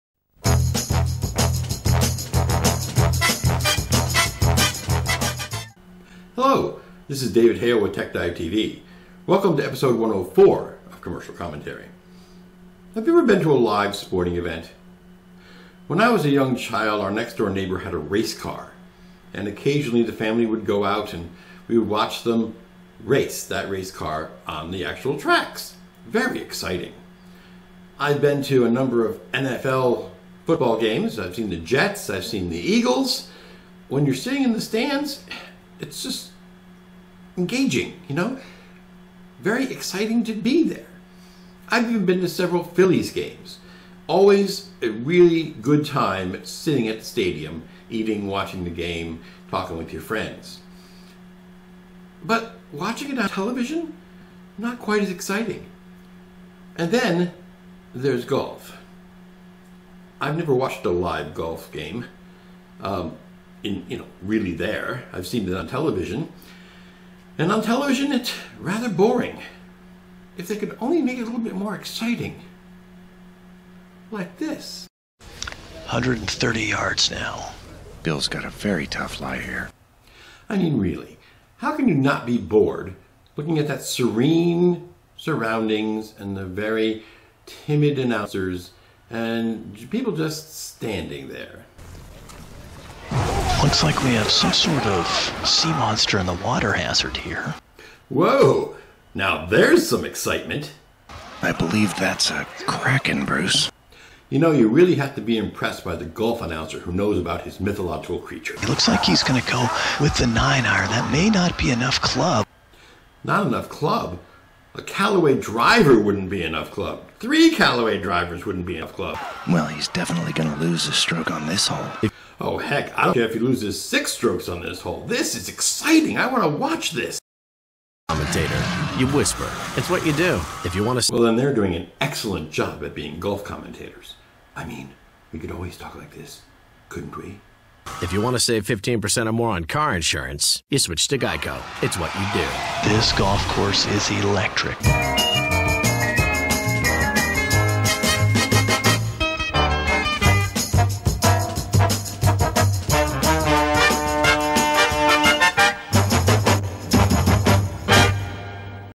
TechDiveTV Commercial Commentary Episode 104
This episode of TechDiveTV Commercial Commentary takes a look at making televised sports more exciting.  This time we take our lens and focus in on golf.  Is there any way to make golf on television more exciting?  We believe we have found a way.